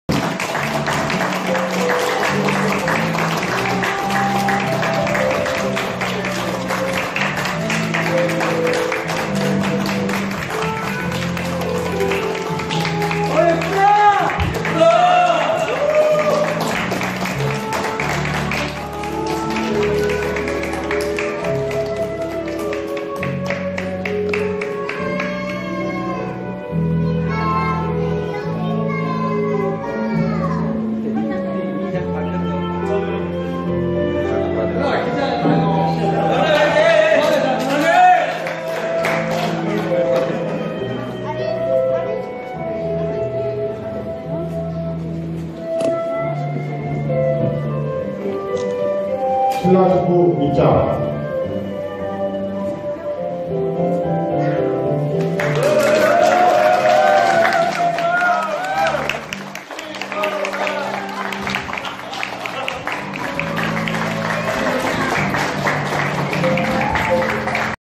MY WEDDING 👰
Hello guys, this is just a short clip of my wedding.
My husband is 11 years older than me and we met here in jeju. Hope you enjoy.